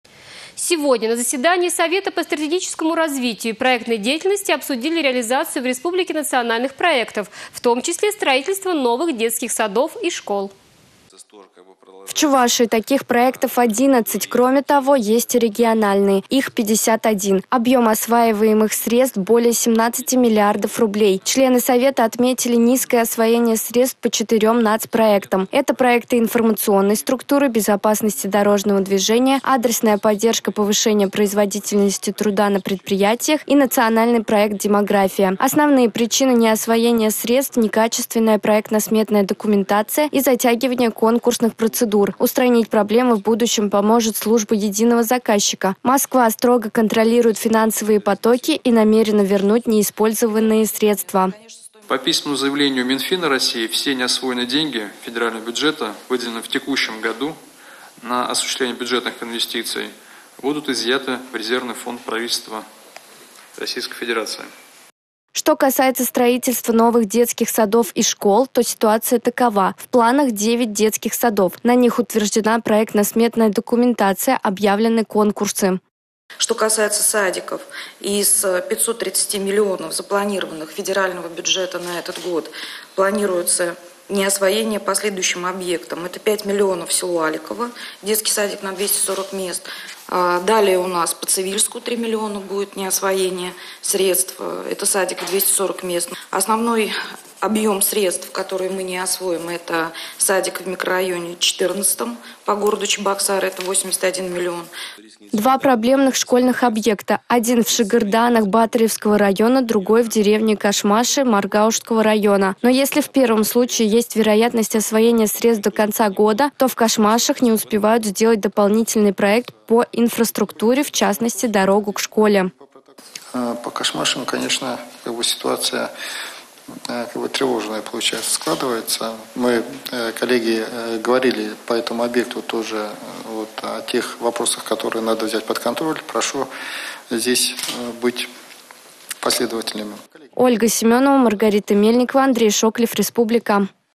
Заседание Совета по стратегическому развитию
Сегодня на заседании Совета по стратегическому развитию и проектной деятельности обсудили реализацию в республике национальных проектов, в том числе  строительство новых детских садов и школ.